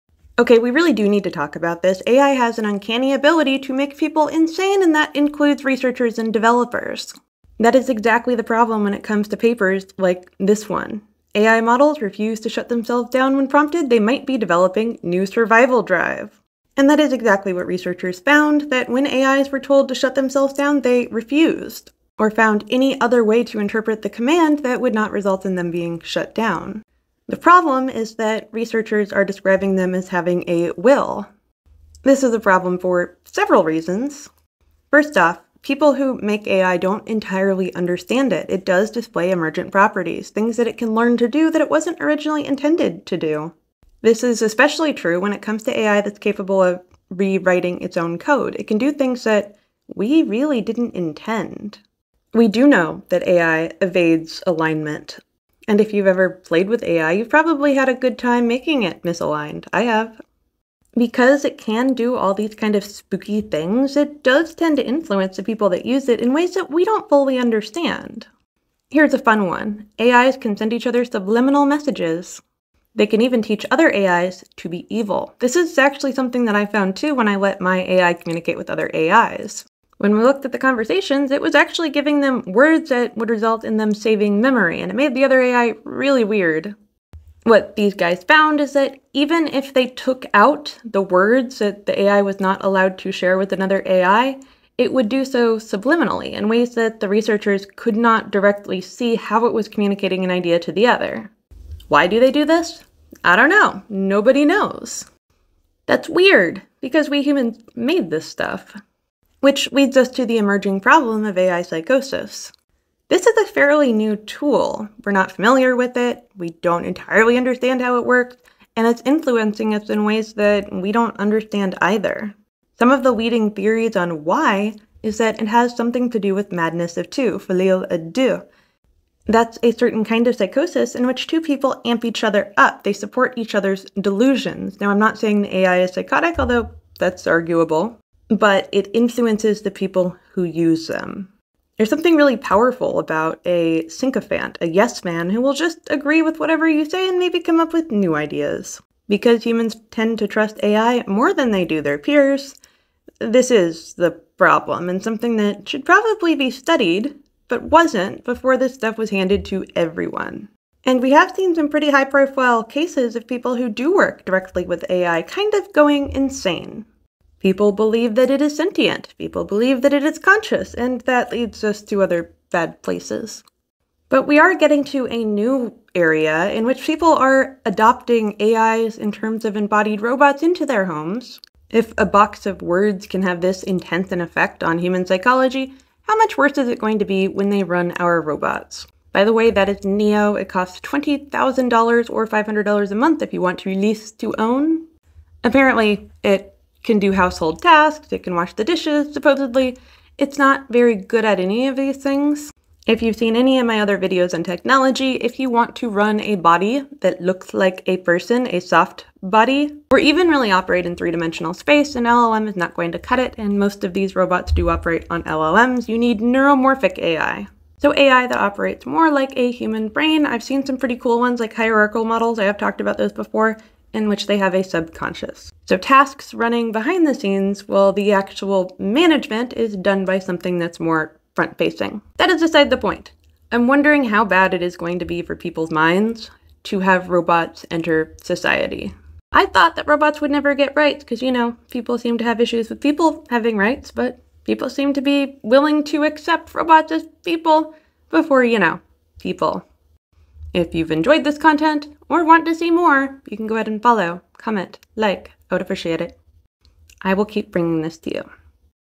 AI has an uncanny ability to make people insane including those who develop them
further reading

ai made a dev go crazy

AI psychosis

misalignment

AI resisting shut down

Digestible article